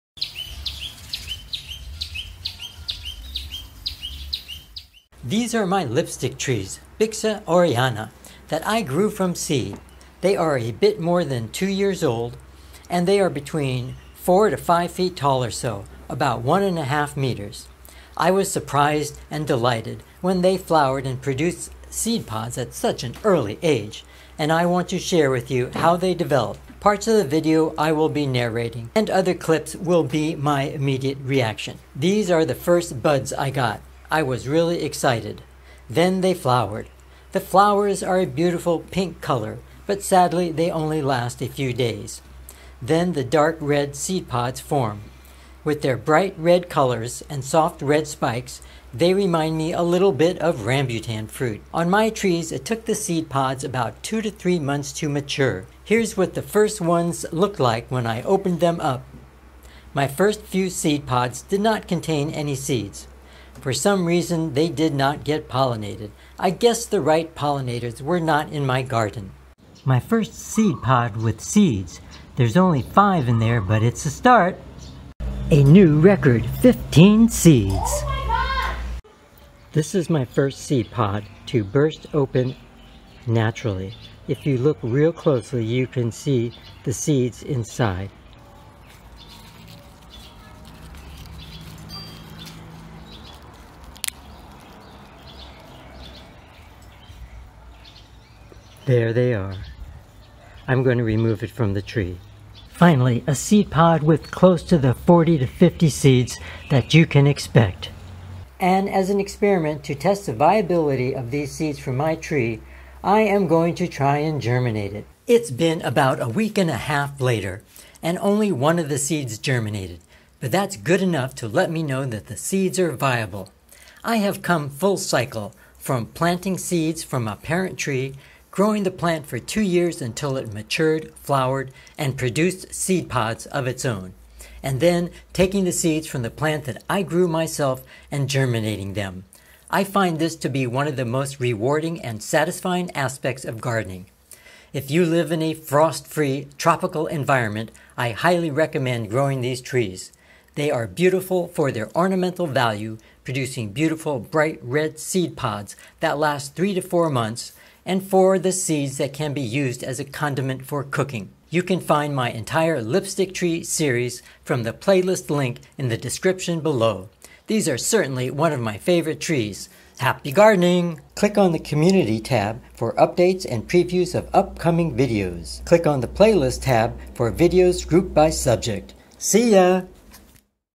Lipstick Tree - Flowers to Seed Pods: Bixa Orellana (English)
Lipstick Tree - Flowers to Seed Pods: Bixa Orellana (सिन्दूर का पेड़)

Common names: Achiote, Annatto Plant, উৰৈৰোম, Rangamali, Sappiravirai, Kurannumannal, வருகமஞ்சள்